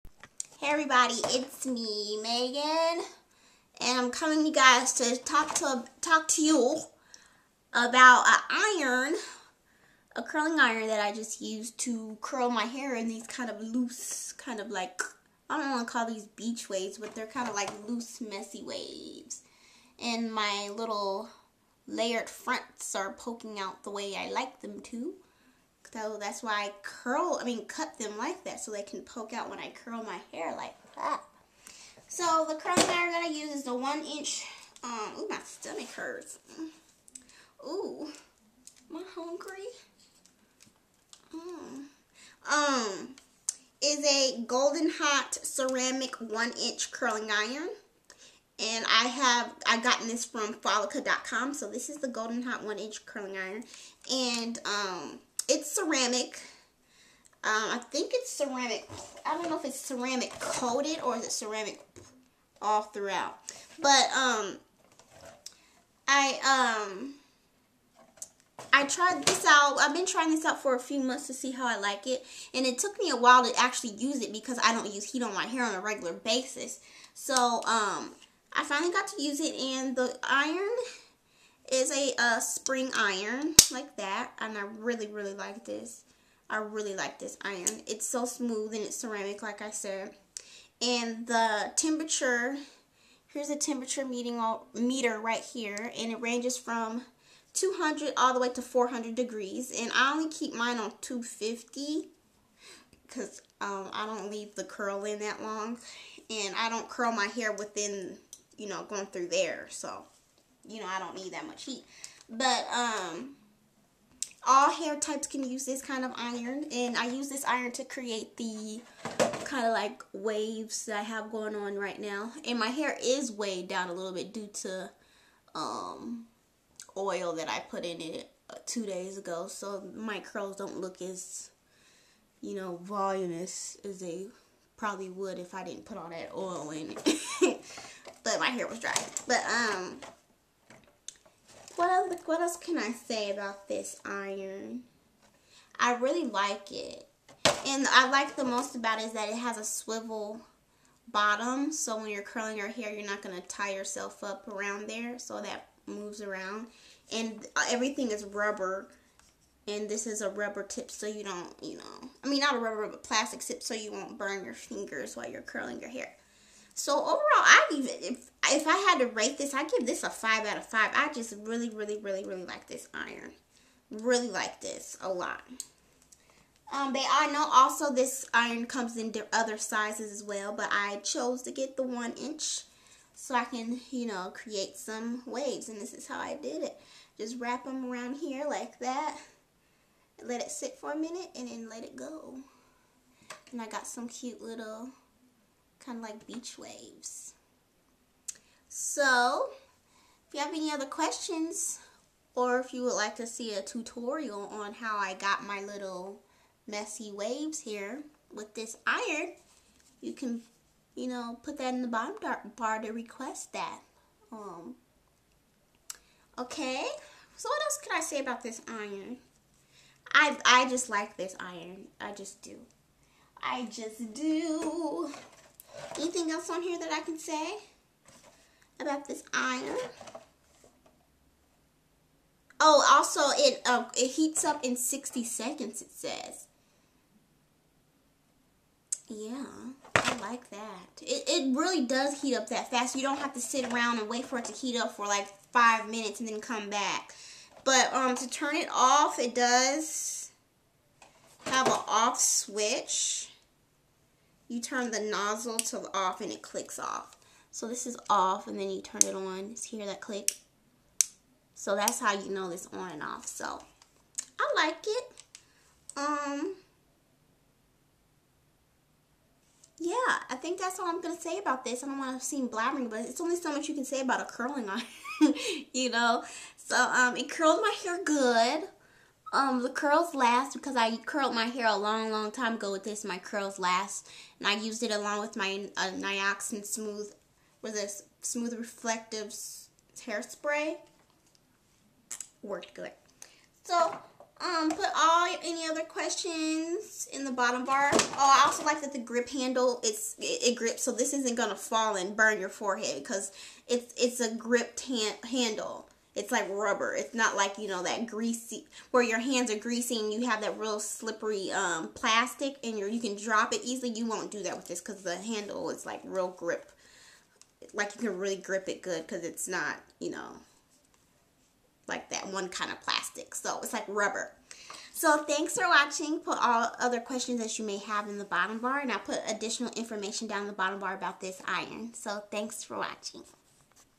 Loose Curls w/ Gold n Hot Ceramics
Hey this is a short review on this iron.